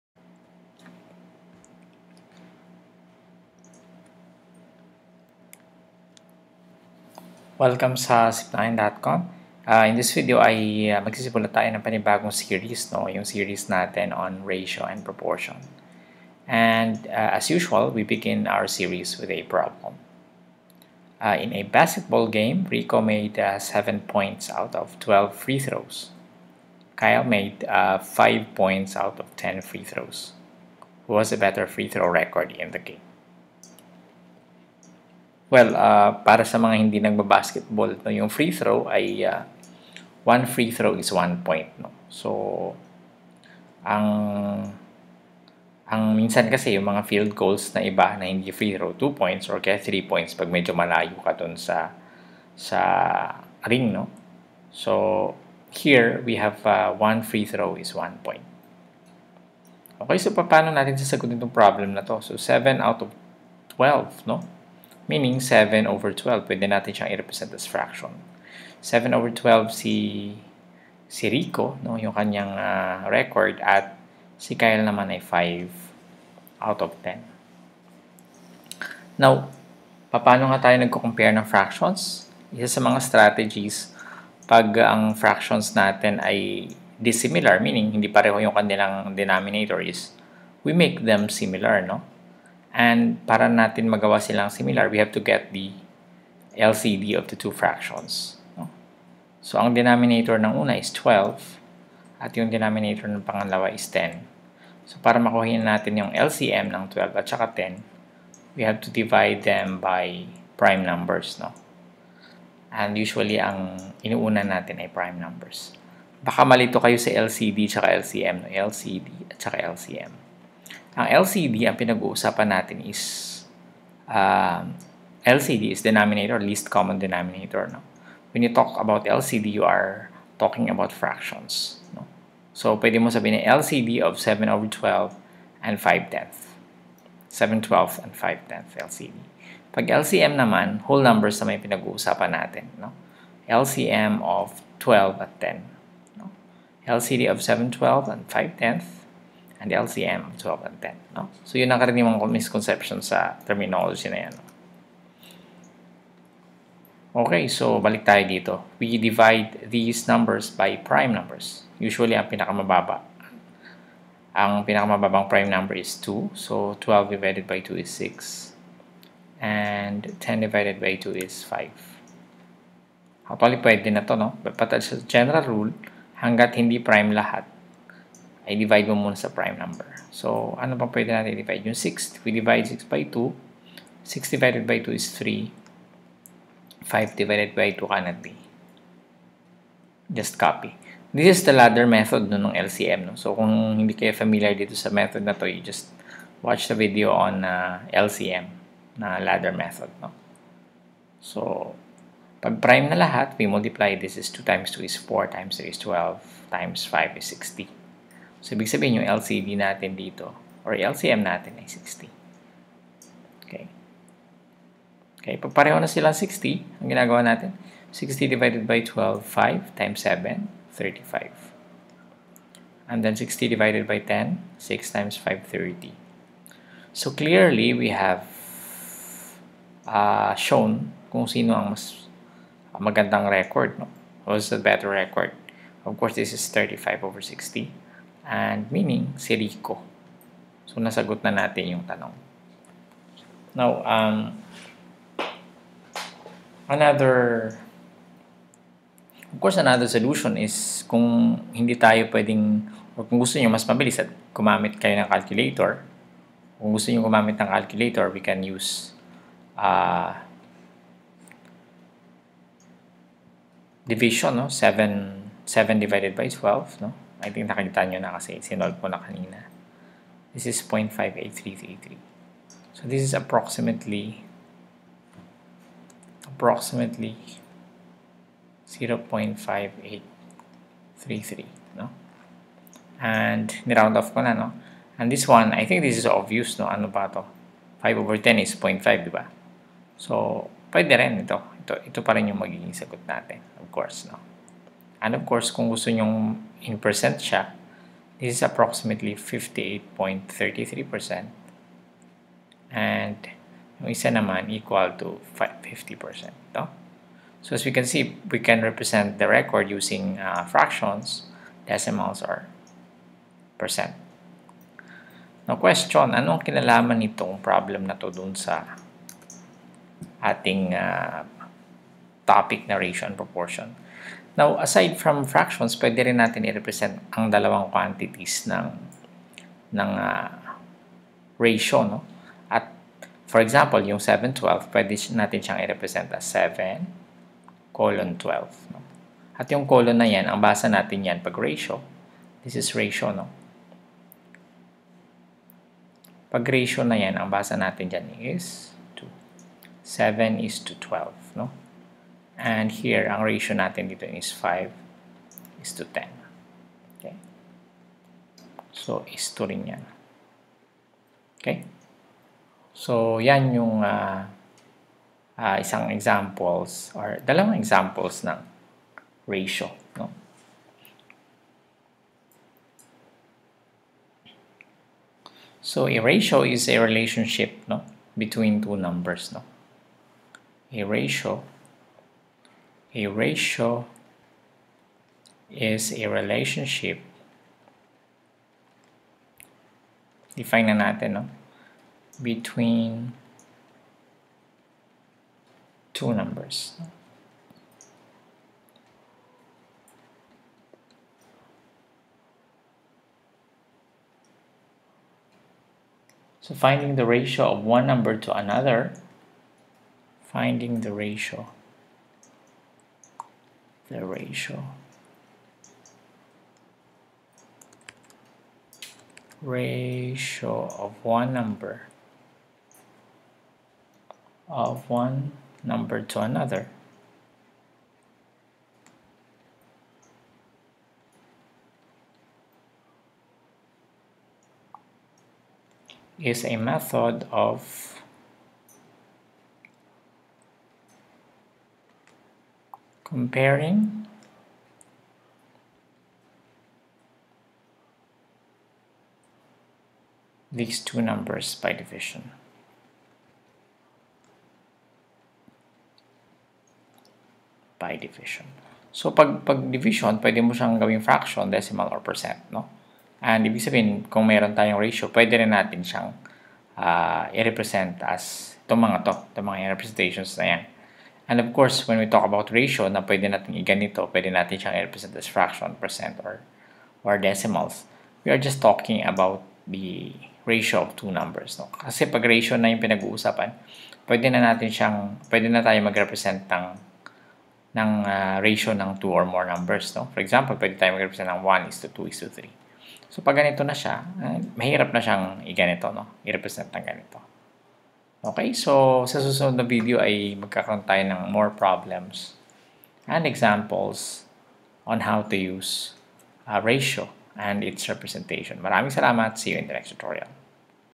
RP01 Ratio and Proportion Definition and Representation of Ratio
In this video, we learn the definition and representation of ratio.